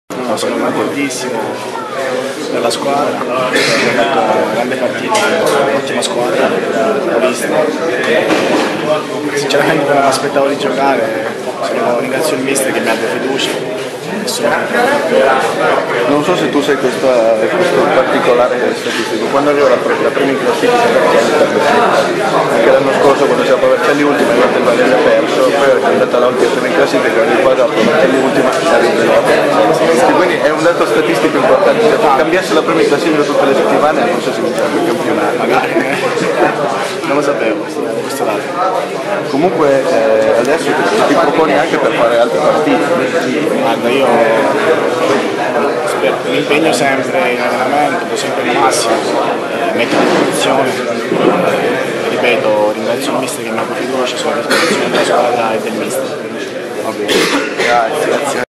03 Pro Vercelli-Spezia 1-0 campionato 2009/10
Il difensore Stefano Prizio intervistato a fine partita in sala stampa